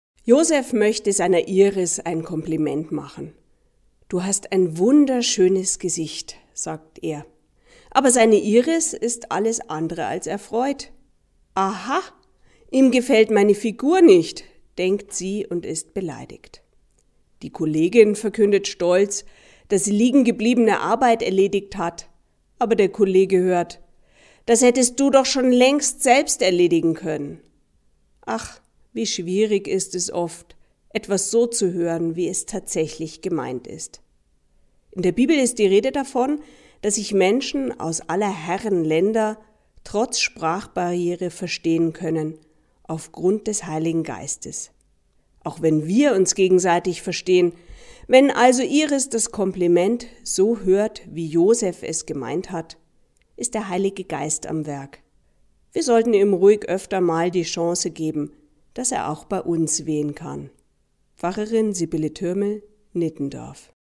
„Komplizierte Komplimente“ von Sibylle Thürmel (23. Okt. 2017)
Josef möchte seiner Iris ein Kompliment machen: „Du hast ein wunderschönes Gesicht“, sagt er. Aber seine Iris ist alles andere als erfreut: „Aha! ….“